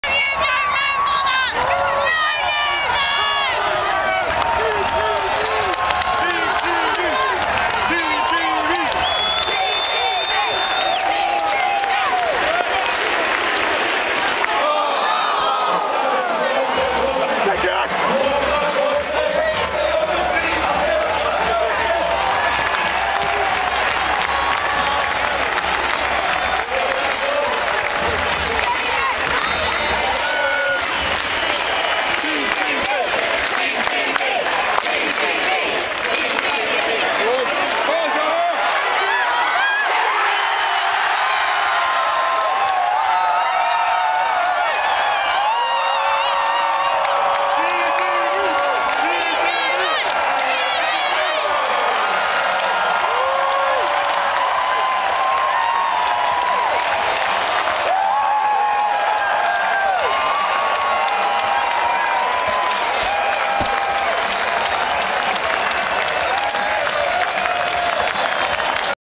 Olympic Beach Volley Ball
Olympic Beach Volley Ball at Horse Guards Parade. Fantastic atmosphere as Team GB was playing.